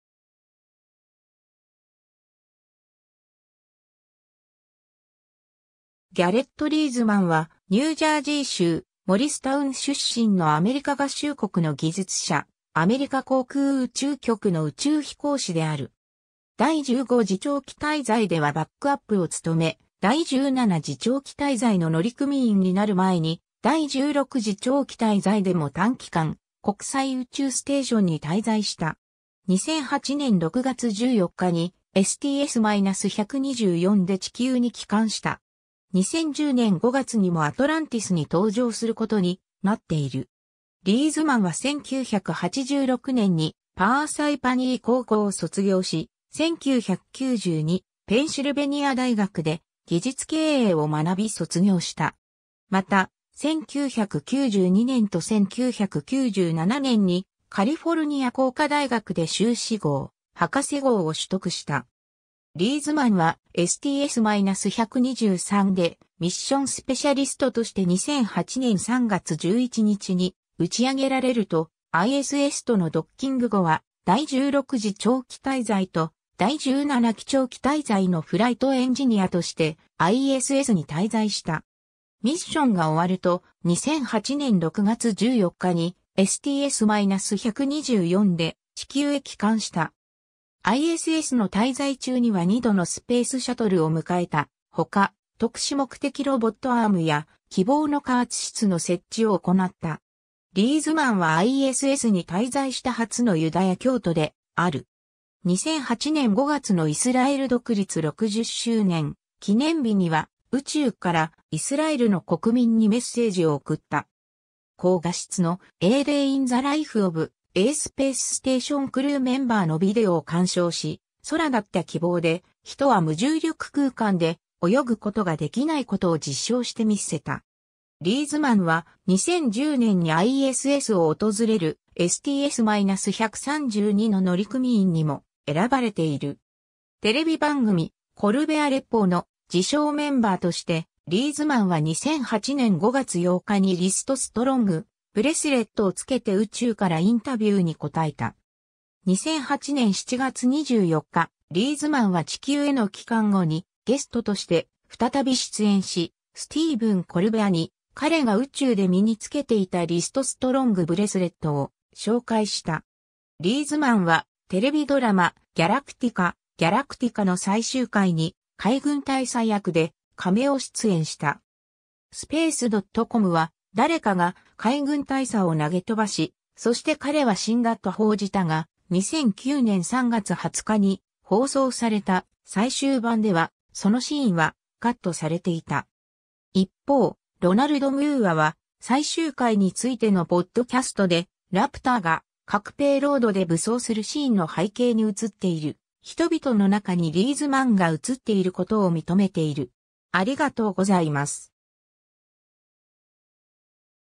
ギャレット・リーズマン
ギャレット・リーズマン（Garrett Erin Reisman、1968年2月10日 - ）は、ニュージャージー州モリスタウン出身のアメリカ合衆国の技術者、アメリカ航空宇宙局の宇宙飛行士である。
 第15次長期滞在ではバックアップを務め、第17次長期滞在の乗組員になる前に第16次長期滞在でも短期間、国際宇宙ステーションに滞在した。
2008年6月14日にSTS-124で地球に帰還した。
2010年5月にもアトランティスに搭乗することになっている。
 リーズマンは1986年にパーサイパニー高校を卒業し、1990にペンシルベニア大学で技術経営を学び卒業した。
また、1992年と1997年にカリフォルニア工科大学で修士号、博士号を取得した。
 リーズマンはSTS-123でミッションスペシャリストとして2008年3月11日に打ち上げられると、ISSとのドッキング後は第16次長期滞在と第17期長期滞在のフライトエンジニアとしてISSに滞在した。
ミッションが終わると、2008年6月14日にSTS-124で地球へ帰還した。
ISSの滞在中には2度のスペースシャトルを迎えたほか、特殊目的ロボットアームやきぼうの加圧室の設置を行った。
 リーズマンはISSに滞在した初のユダヤ教徒である。
2008年5月のイスラエル独立60周年記念日には、宇宙からイスラエルの国民にメッセージを送った。
高画質の'A day in the life of a space station crew member'のビデオを鑑賞し、空だったきぼうで人は無重力空間で「泳ぐ」ことができないことを実証してみせた。
 リーズマンは、2010年にISSを訪れるSTS-132の乗組員にも選ばれている。
 テレビ番組『コルベア・レポー』の自称メンバーとして、リーズマンは2008年5月8日にリスト・ストロング・ブレスレットを付けて宇宙からインタビューに答えた。
2008年7月24日、リーズマンは地球への帰還後にゲストとして再び出演し、スティーヴン・コルベアに彼が宇宙で身に付けていたリスト・ストロング・ブレスレットを紹介した。
 リーズマンはテレビドラマ『GALACTICA/ギャラクティカ』の最終回に海軍大佐役でカメオ出演した。
Space.comは「誰かが海軍大佐を投げ飛ばし、そして彼は死んだ」と報じたが、2009年3月20日に放送された最終版では、そのシーンはカットされていた。
一方、ロナルド・ムーアは最終回についてのポッドキャストで、ラプターが核ペイロードで武装するシーンの背景に映っている人々の中にリーズマンが映っていることを認めている。